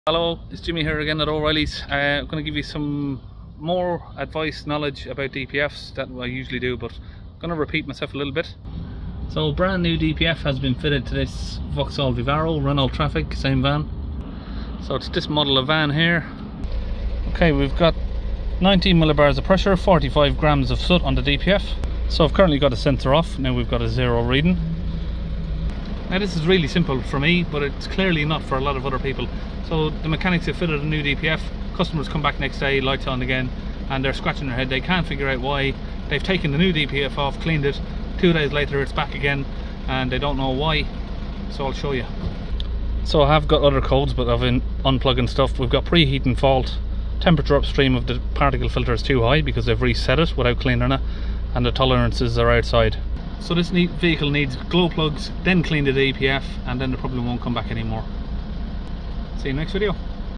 Why Is My Brand New DPF Blocked ?
brand new DPF Diesel Particulate Filter blocked 24 hours after fitting it to the vehicle?

diagnostic assessment tests to find out the cause of the fault before fixing it